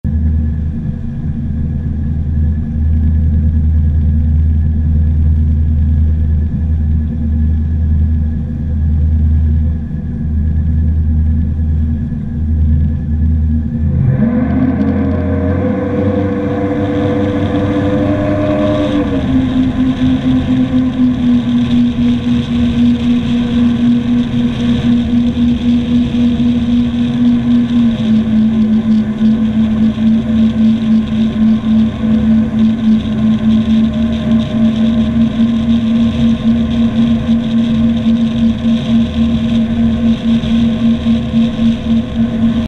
Yamaha 212X planing
Just a video I wanted to show someone. I wasn't trying to get on plane really quick, this is just the only footage I got of a holeshot this weekend.